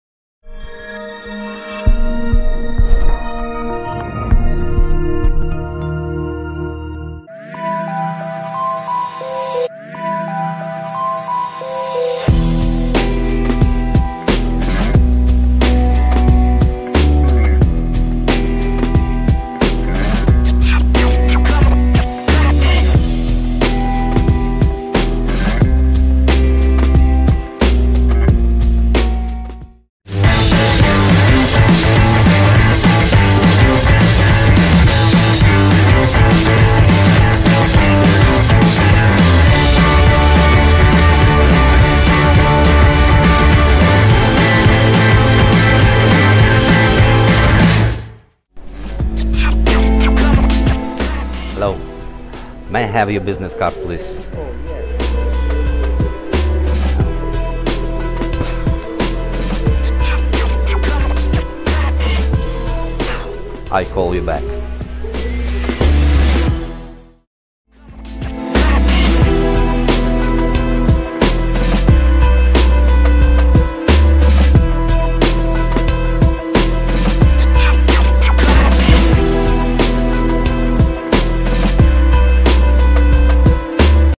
BurnOUT Victor Voitko demo
Viktor a ajouté sa touche professionnelle et artistique à un accessoire classique. Il est l'un des artisans les plus occupés dans l'art de la magie voyageant dans le monde année après année. Il a une excellente compréhension de la magie et des accessoires magiques. L'adaptation récente de cet effet par Viktor se traduit par une amélioration spectaculaire de tout ce qui l'a perçu auparavant.

Lorsqu'il s'agit de faire une impression durable, aucun appareil ou gimmick n'est plus mémorable que le FEU ! Par exemple, distribuer votre carte de visite qui, quelques instants auparavant, était en feu ! Idem pour un billet de banque ! D'autres applications sont possibles, limitées à la fois à l'imagination et à certains éléments qui fonctionneront avec ce nouvel appareil.